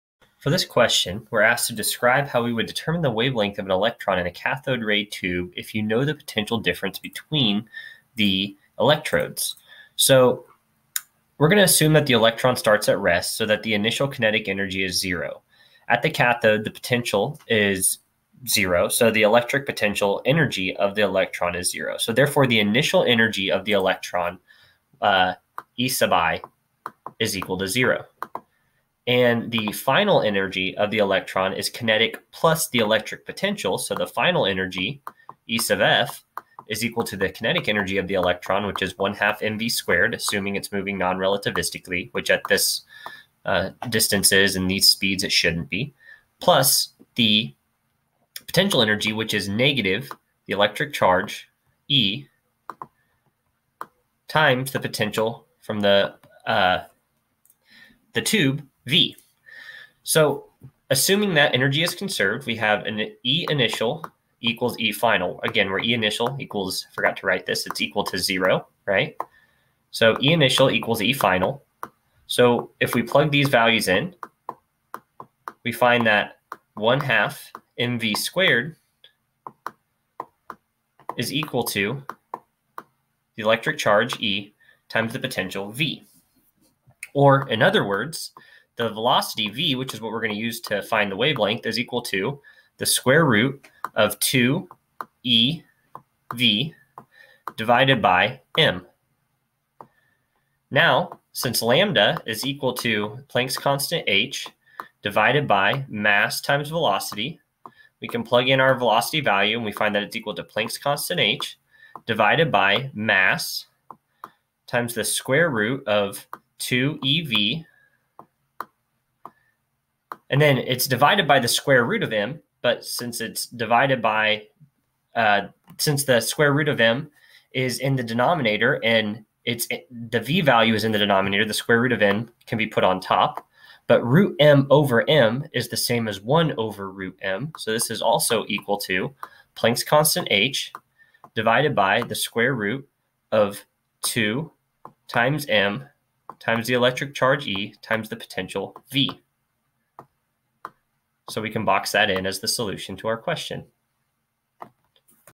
Describe how you will determine the wavelength of an electron in a cathode ray tube if you know the
Describe how you will determine the wavelength of an electron in a cathode ray tube if you know the potential difference between the electrodes.